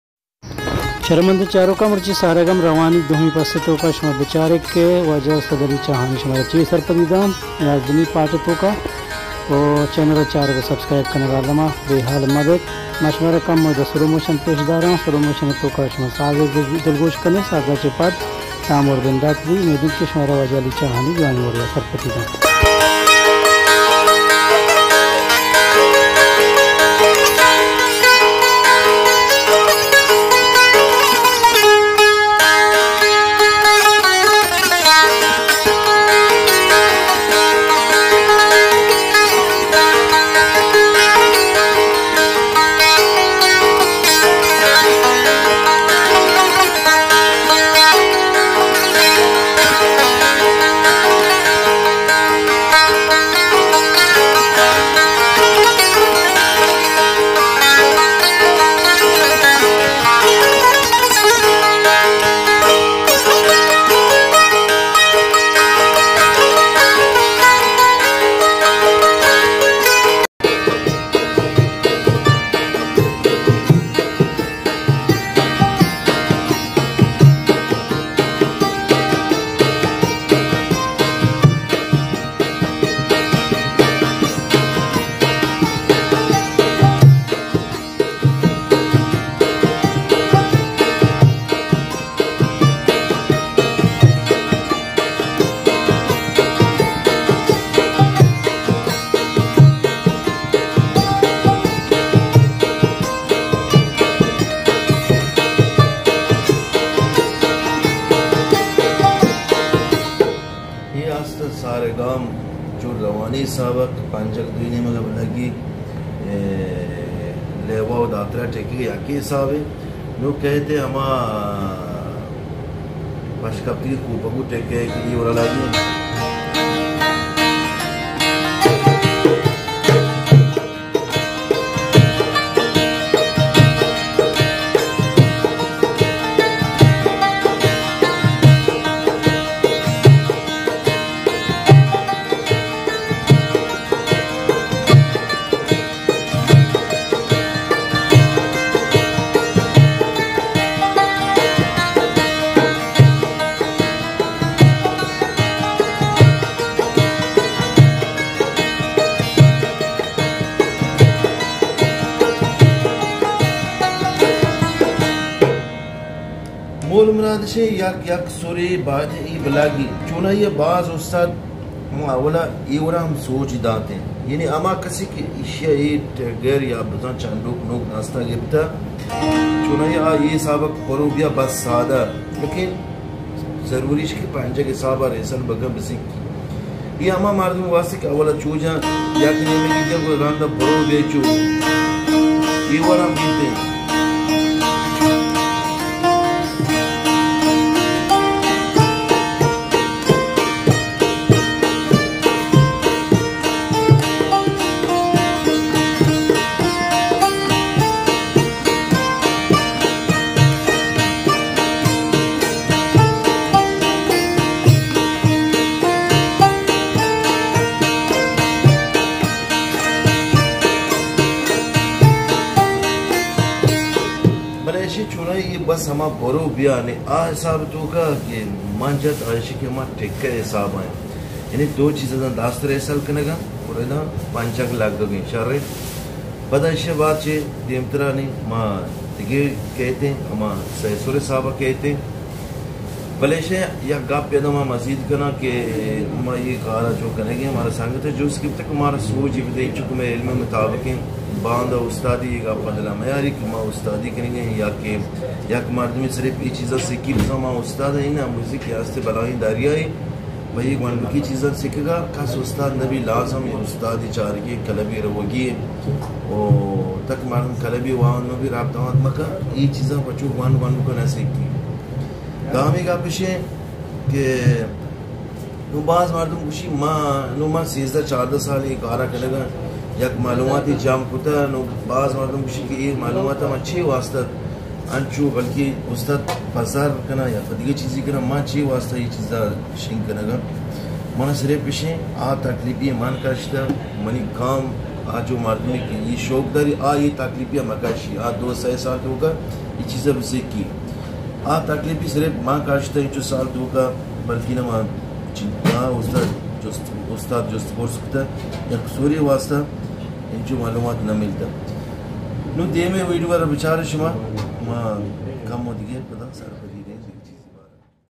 Sargam Rawani Dowmi Pasth P11 Ali Chahani Baloch sach Balochi سارگم روانی دومی پست
سارگم روانی دومی پست
سرھال بینجو
استاد علی چاھانی
تبلہ ءِ سرا مقبول مھر بلوچ
Sargam rawani Dowmi Pasth
topic  Banjo instruments  Balochi language
Ustad Ali chahani Baloch
Part 11